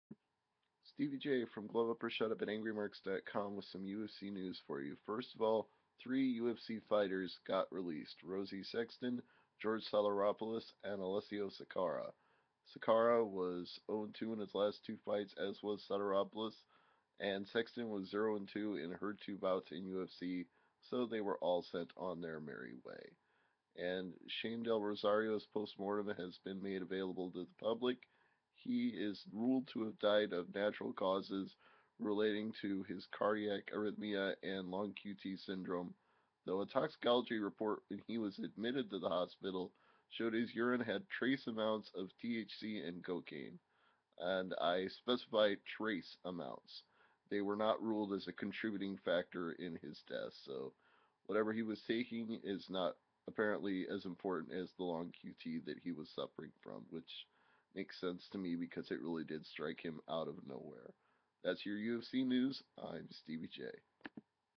George Sotiropoulos, Alessio Sakara & Rosi Sexton Released + Shane Del Rosario News
or Shut Up's Stevie J with some UFC news on a Friday night. George Sotiropoulos, Alessio Sakara and Rosi Sexton were all released by the UFC, and Shane Del Rosario's postmortem concludes that he died of natural causes, regardless of the fact his urine sample at the hospital found trace amounts of cocaine in his system.